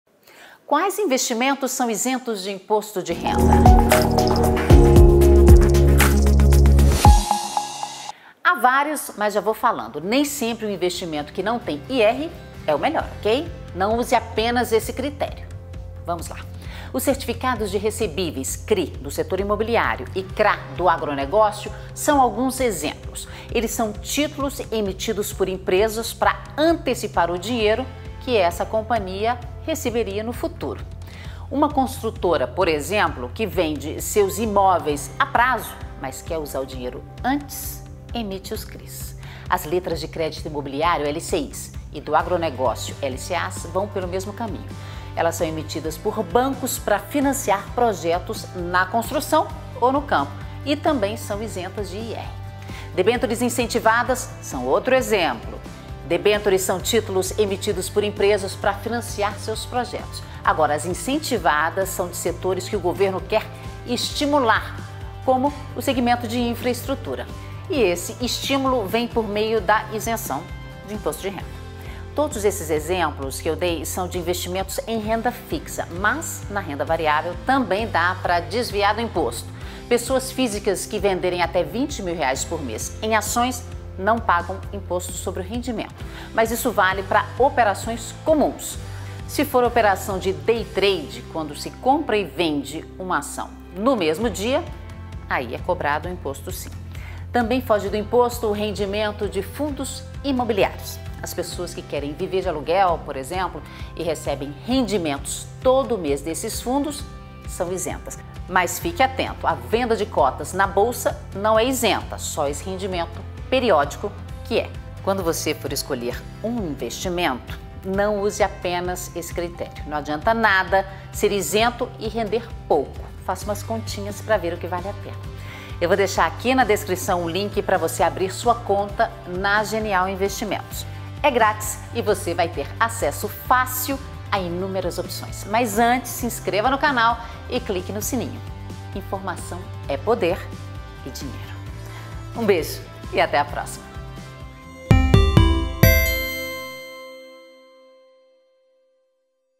Quais investimentos são isentos de imposto de renda?
Atualmente, existem diversos investimentos isentos de Imposto de Renda. Mas atenção: nem sempre aquele que não tem IR é o melhor. No Genial Responde, Denise Barbosa fala mais sobre o tema e indica quais as opções disponíveis no mercado.

PS. Ganhe 1 curso "Como começar a Investir" abrindo sua conta HOJE!